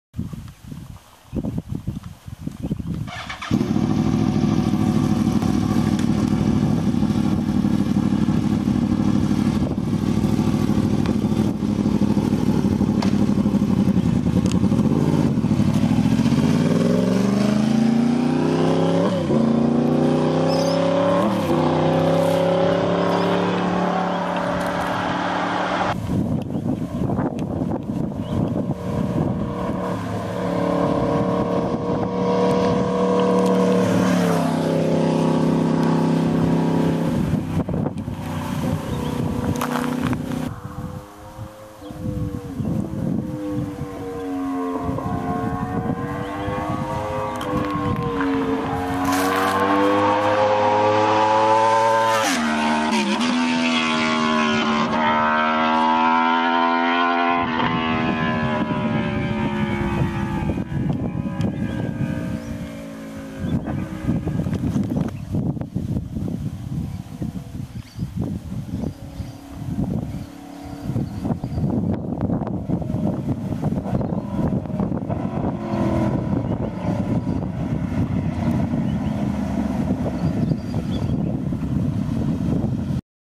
Bigbang R1 no mufflers
2010 r1 crossplane bigbang with the mufflers removed but still retaining factory headers and cats , start up, normal ride by and then a fly by.